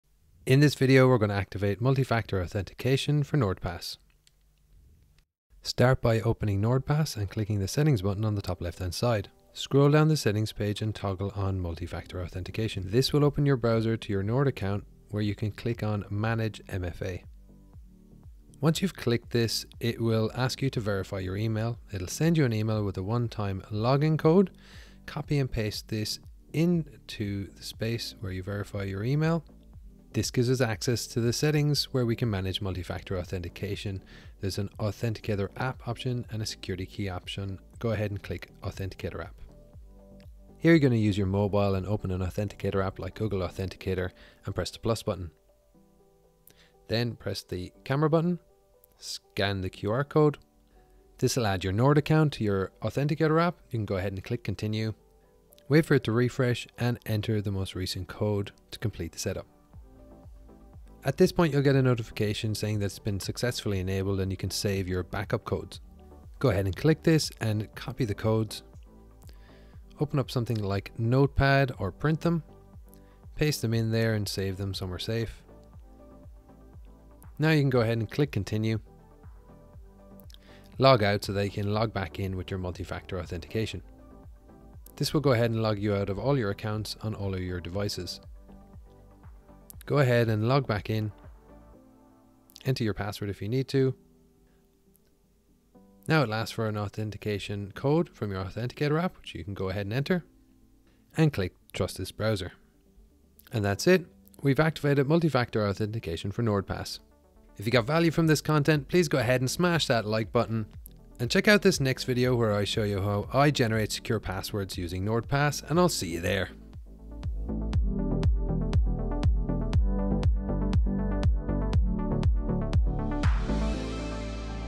Enable Multifactor Authentication for NordPass: Quick Guide for Beginners
Quick and easy guide for beginners on how to enable Multifactor Authentications / Two factor authentication for NordPass password manager

📒 Show Notes  📒

💥👇 Three discounted tools I use to protect me and my family!👇💥

QUESTION — Did this tutorial help you? Post in the comments section of this video!

About:
This is a quick 2 minute tutorial on how to enable enable Multifactor Authentications / Two factor authentication for NordPass password manager using a mobile Authenticator app.